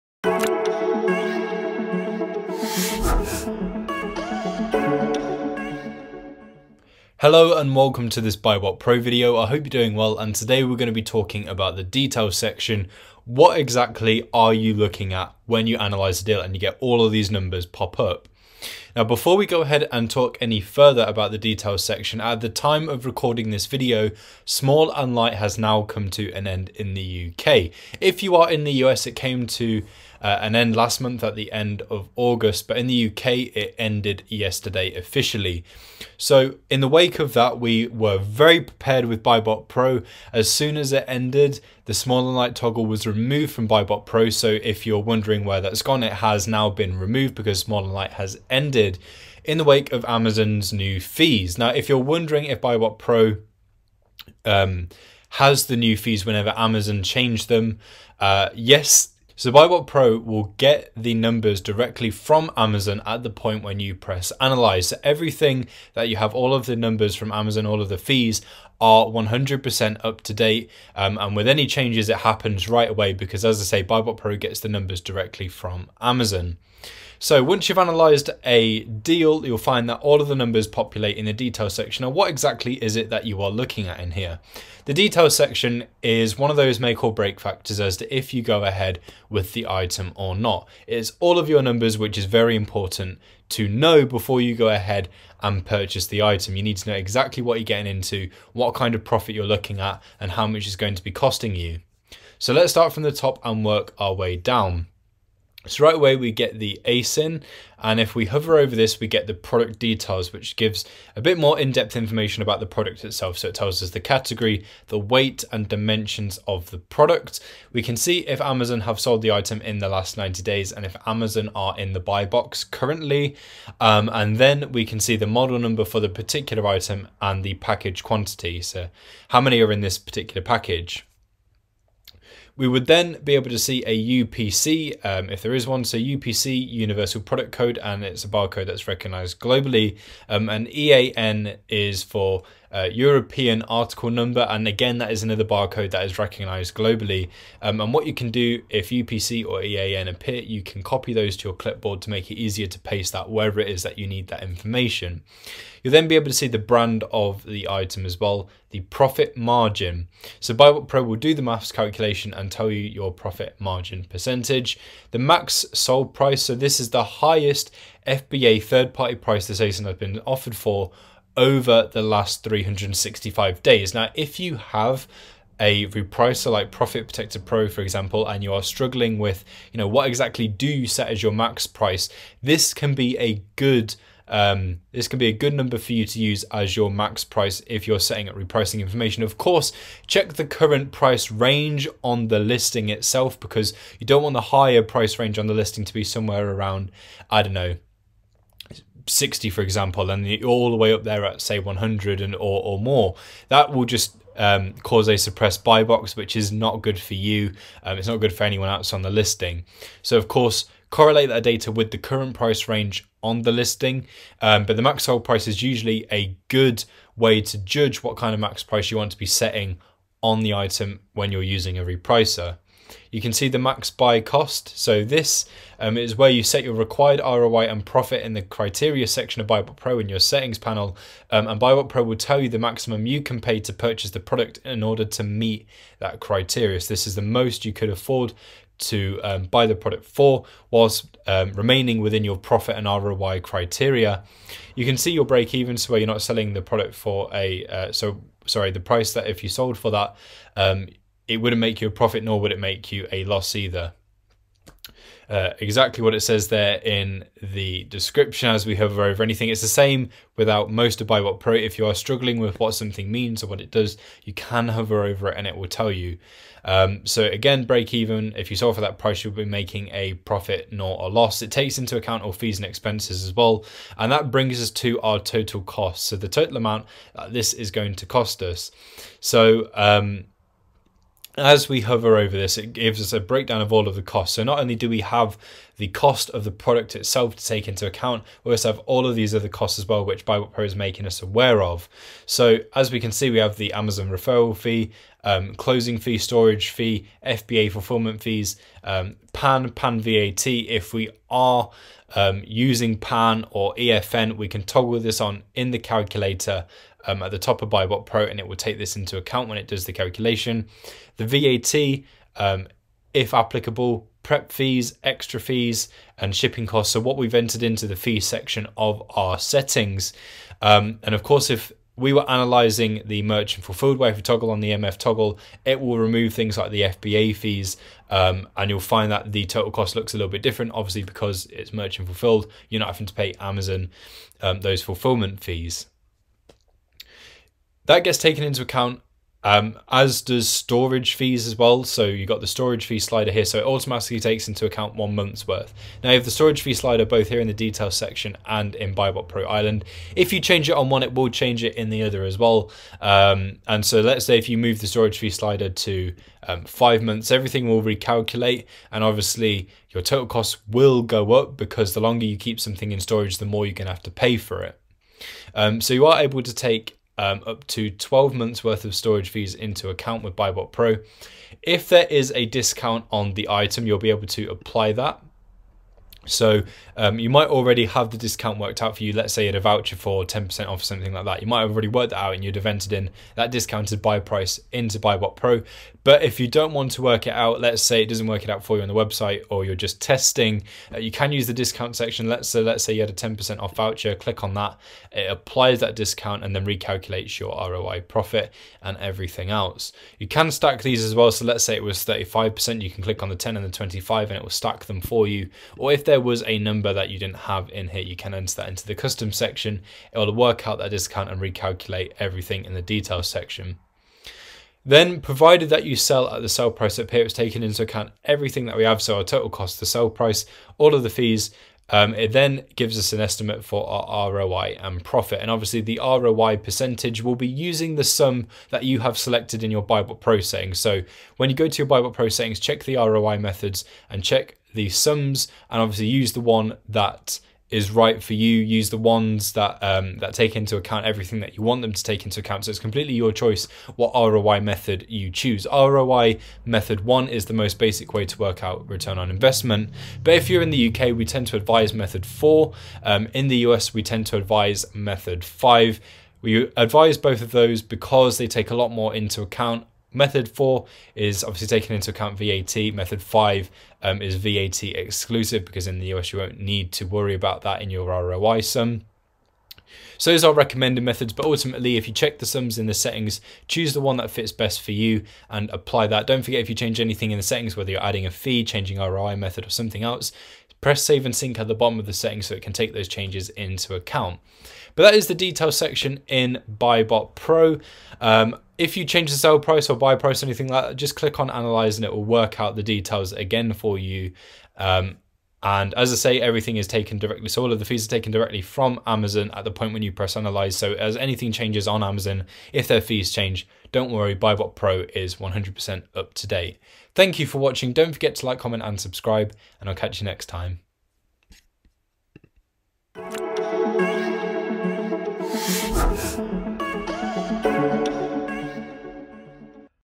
Find Out Your Profit, ROI, Amazon Fees, Break Even And More In BuyBotPro's Details Section!
The Details section is the home of all of your important numbers, and will be one of the main dictating factors as to if you can proceed with a deal on Amazon or not.

In this video, we'll decipher the jargon and show you where you can find further product details and a total costs breakdown.